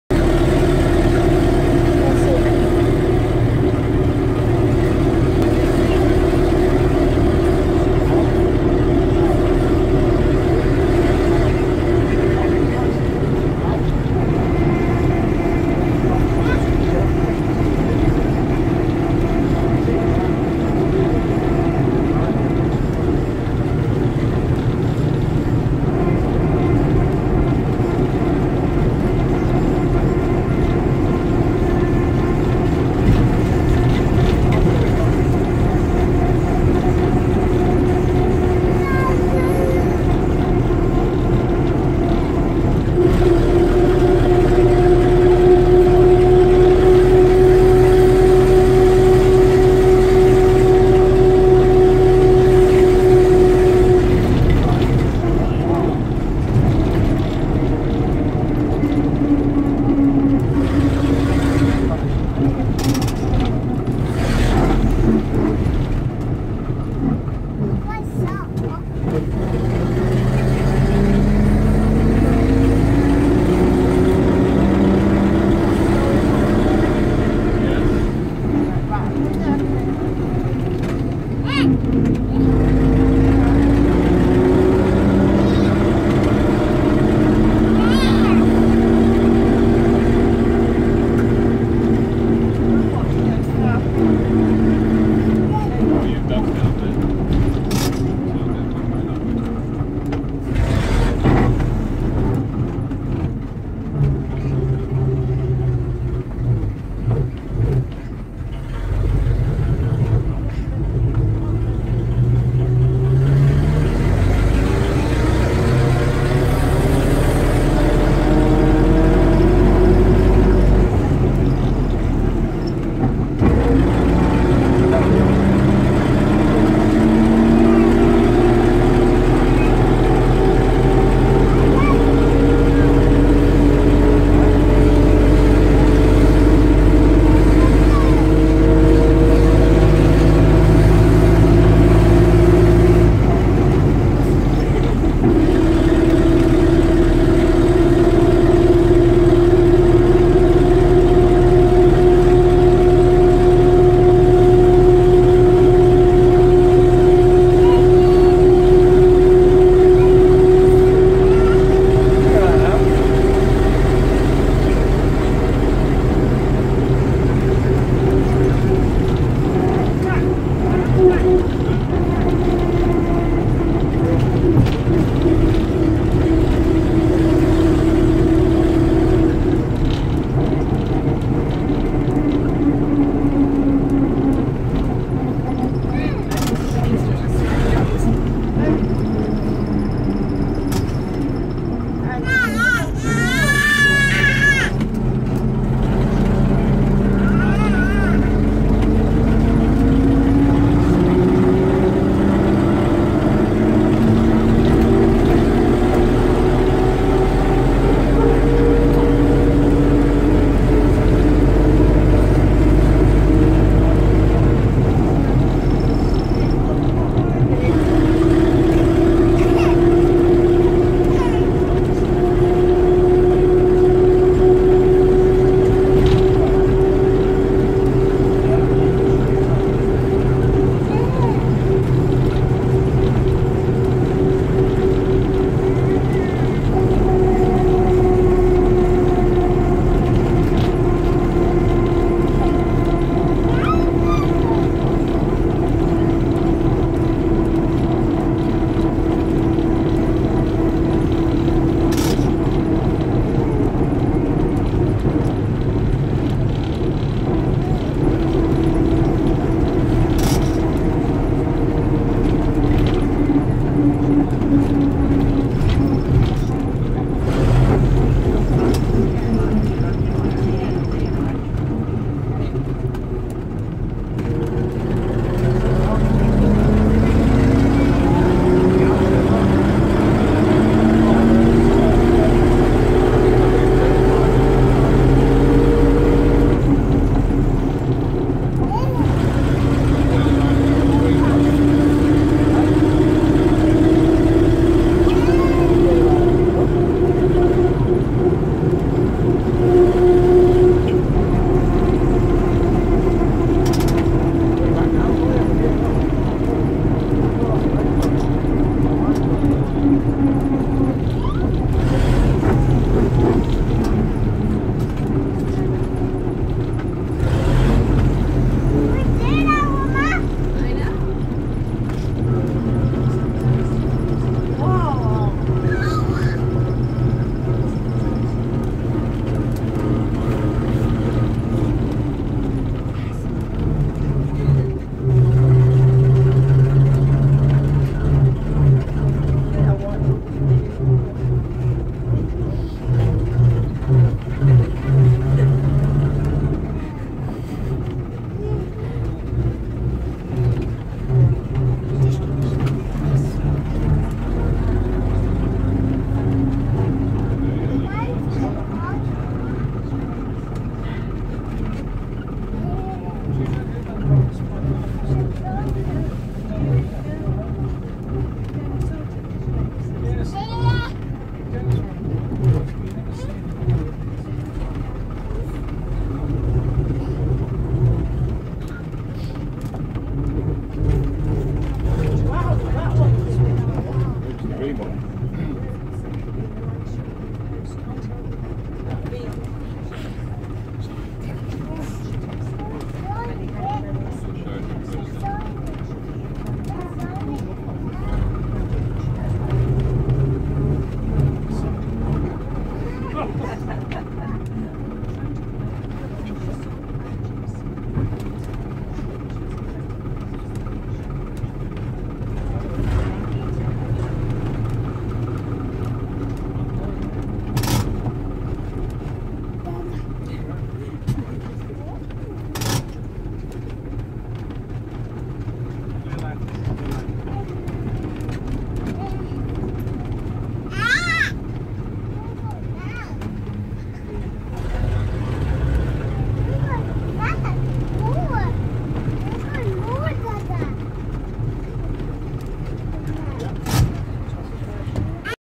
(HD) A trip on a Leyland Lioness Charabanc 19/9/2016
Today at the Autumn Transport Show in Leyland the British Commercial Vehicle Museum owned Leyland Lioness Charabanc was providing free 20 minute circular tours around Leyland for as part of a day out for the members of public that were in attendance at the show today